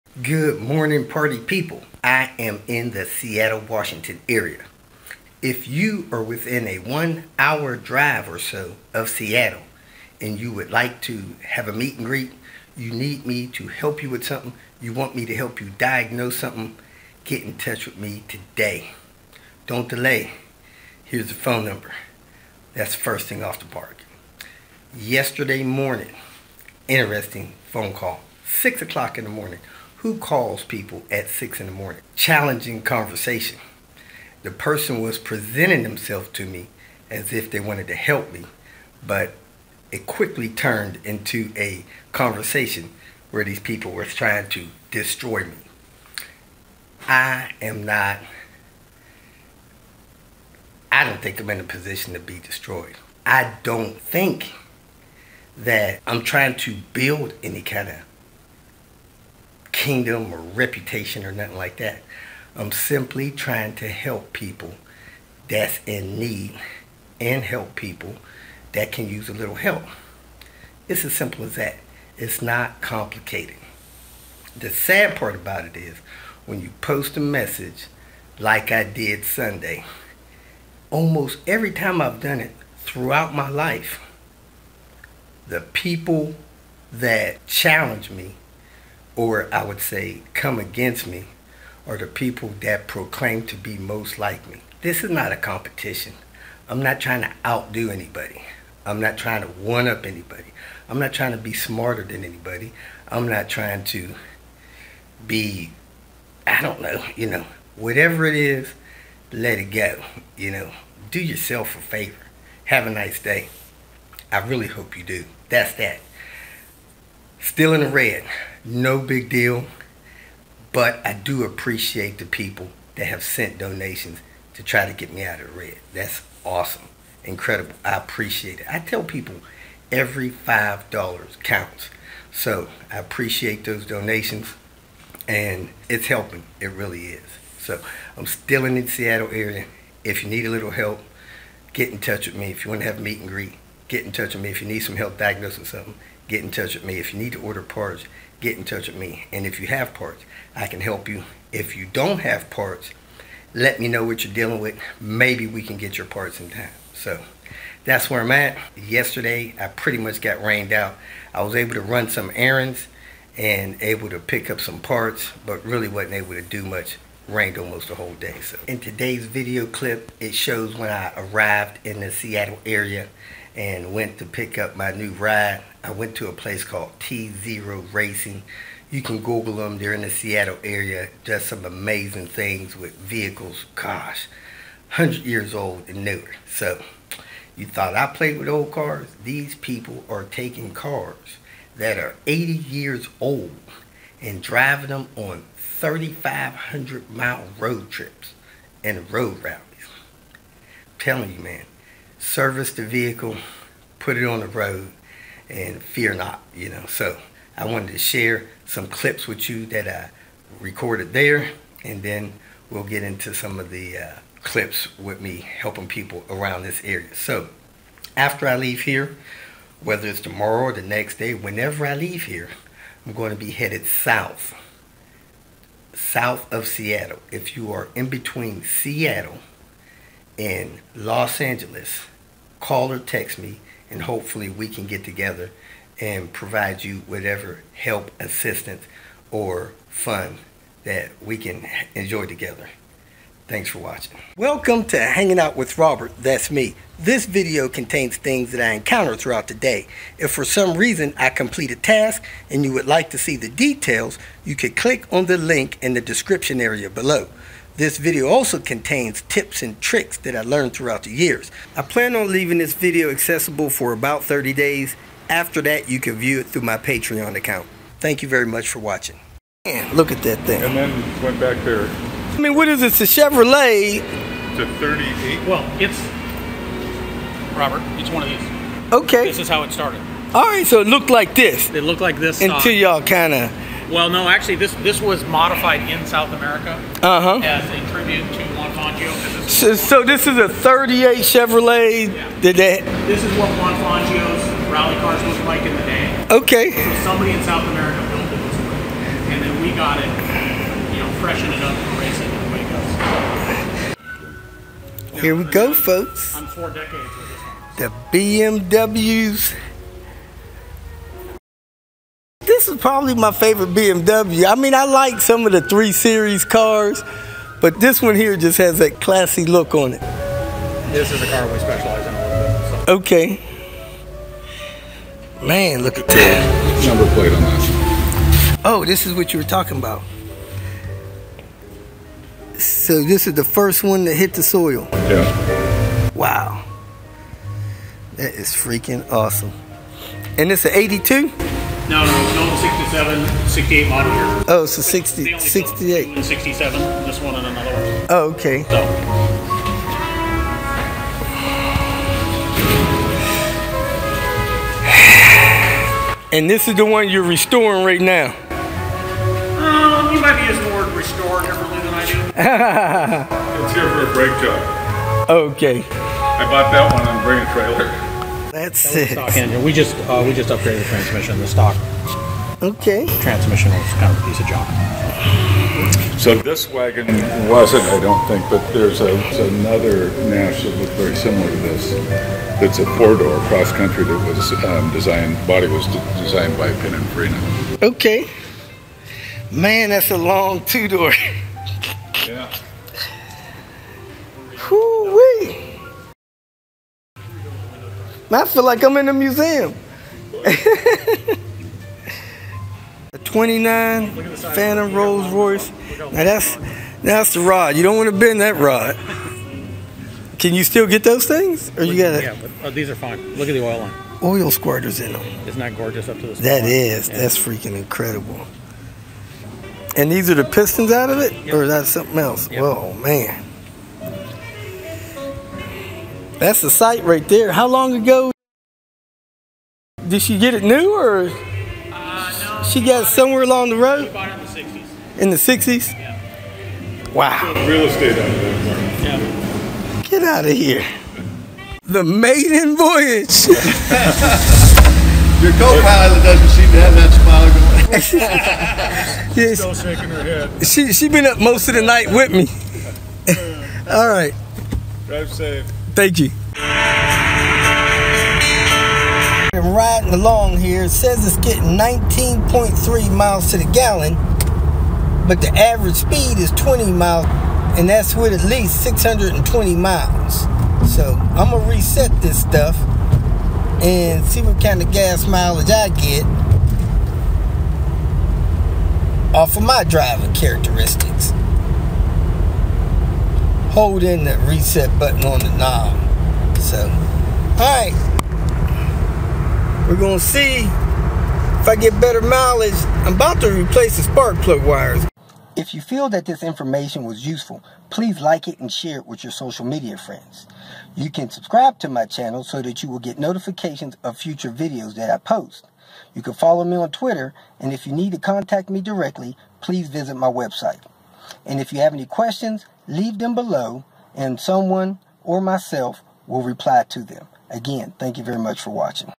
Now in Seattle WA, Portland OR, Eugene OR, etc. Picked up my new ride at T-Zero Racing.
In this video I share that I am on the road and willing to help, meet or assist others. I also picked up my new ride at T-Zero Racing.